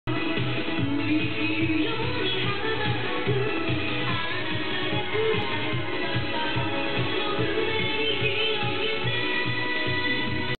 EL Flash T-Shirt - Design 03
This LED EL T-shirt is made of 100% cotton. It has an electro-luminescent EQ meter emblazoned across the front that reacts to and 'pump' with whatever you're listening to. And it also gives a visual rhythm guide to others. It is powered by a battery pack that put inside the T-shirt. Specifications: - Size: M or L - Color: Black - Fabric: Cotton - Power Supply: Standard 4*AAA batteries(not included) - Slide switch to operate power Note: - Please do not bend or press the flash panel - Do not submerge any part of the flash panel - Dry clean or hand wash only - Remove EL panel, battery pack and the whole power cord carefully before washing